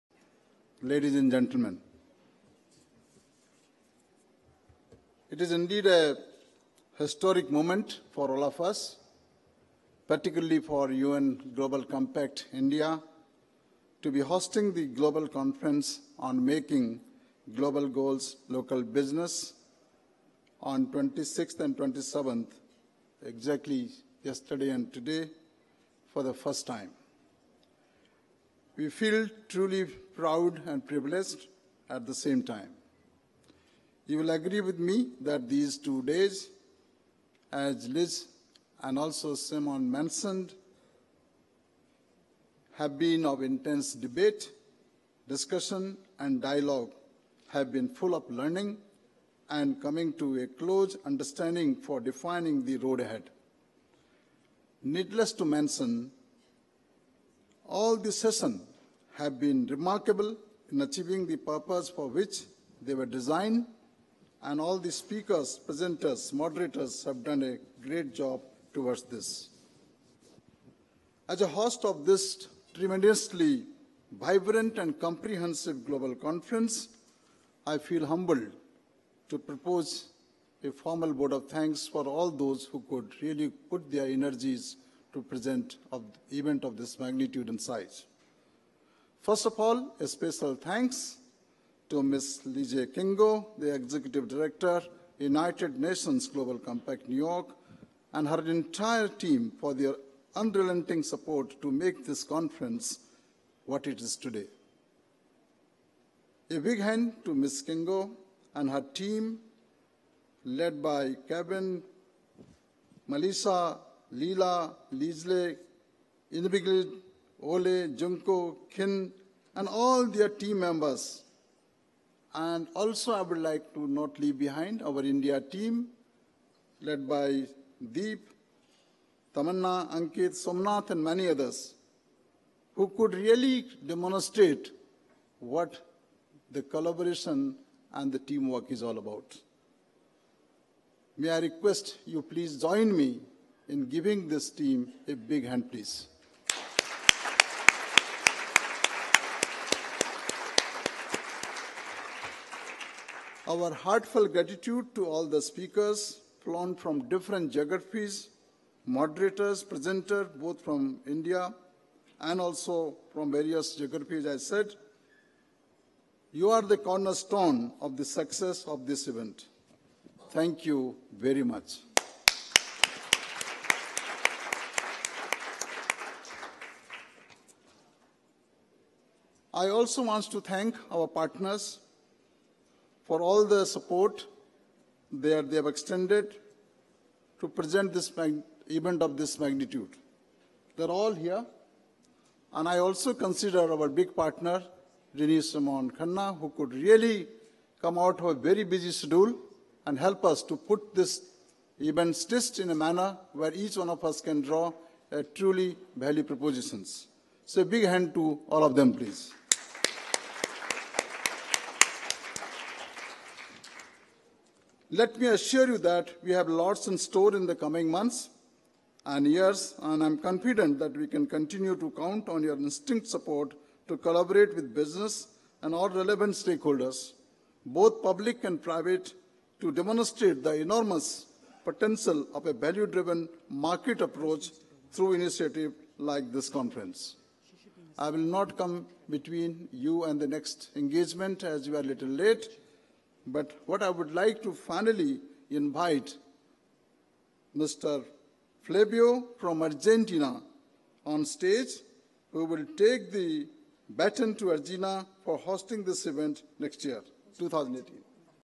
27 April, 2017 - Kamal Singh - Closing Plenary
Learn about the search for the 2017 SDG Pioneers and New York as the next stop on the UN Global Compact's journey.

Speaker: Kamal Singh, Executive Director, Global Compact Network India